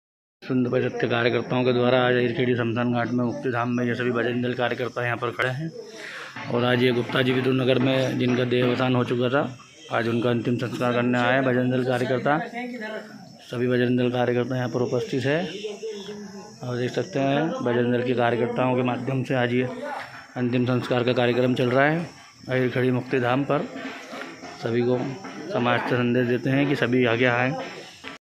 विश्व हिन्दू परिषद् बजरंग दल बना मानवता की मिशाल कराया अंतिम संस्कार
इंदौर शहर के विदुर नगर में कल रात 10 बजे सशिधर गुप्ता नामक व्यक्ति की संधिग्ध परस्थिति में मौत हो गई थी  परिवार में उनकी पत्नी के अलावा ओर कोई नहीं रहता है  वर्तमान कोराना काल में कोई भी रिश्तेदार भी आने को त्यार नहीं था जब रहवासियों ने बजरंग दल जिला संयोजक पप्पू कोचले से संपर्क किया तो उन्होंने द्वारिका पूरी थाना प्रभारी को घटना से अवगत कराया ओर बजरंग दल के  के कार्यकर्ता आकाश ठाकुर पंकज राठौर लक्की राठौर राकेश कोचले ओर अन्य रहवासियों के मिलकर गुप्ता जी के अंतिम संस्कार की व्यवस्था जमाई
परिवार से गोविन्द जी नीमा ओर संतोष जी वाघमारे उपस्थित रहे
जगन्नाथ जिला मंत्री संजय जी चौहान ओर नितिन जी माहेश्वरी ने तुरंत शव वाहन उपलब्ध कराया ओर गो माता के गोबर से बने कंडो से गुप्ता जी का अंतिम संस्कार अहिरखेडी मुक्ति धाम पर किया गया